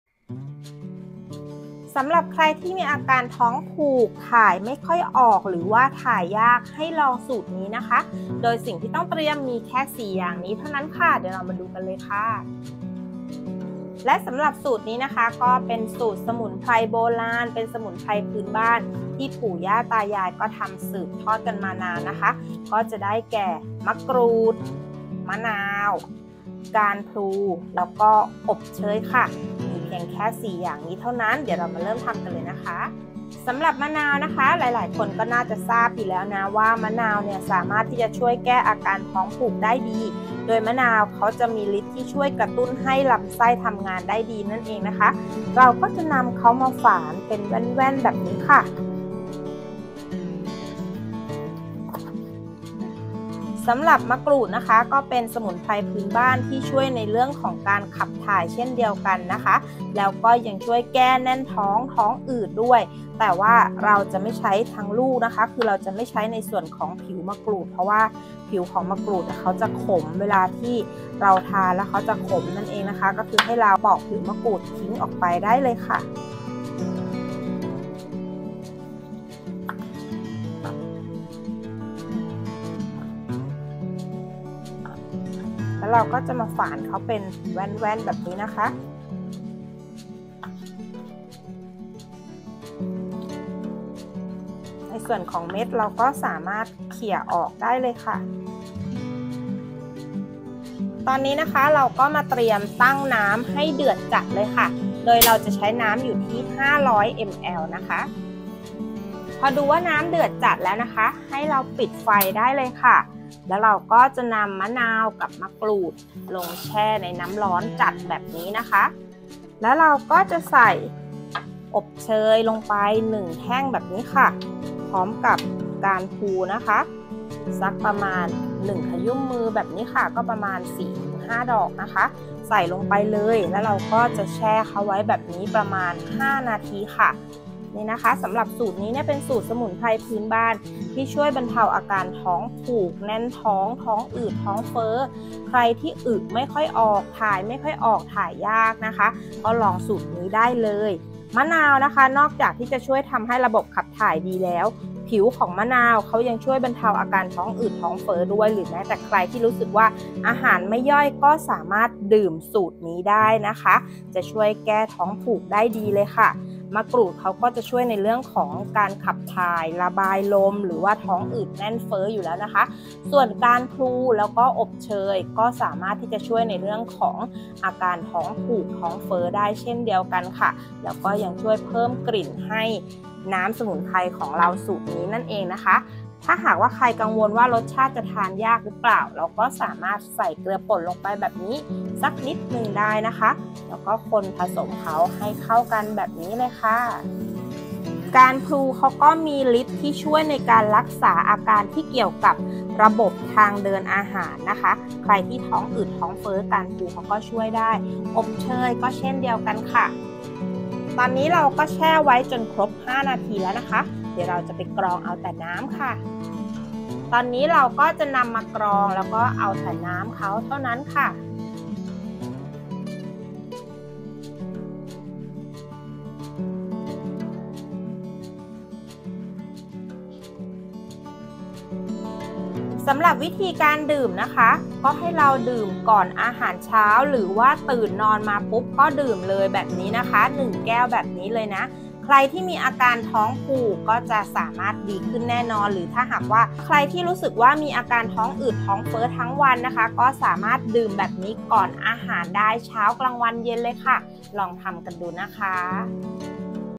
ใครที่มีปัญหา ท้องผูก ถ่ายยาก ฟังทางนี้
ใครที่มีปัญหา ท้องผูก ‼️ ถ่ายไม่ออก แน่นท้อง อาหารไม่ย่อย ต้องลองสูตรนี้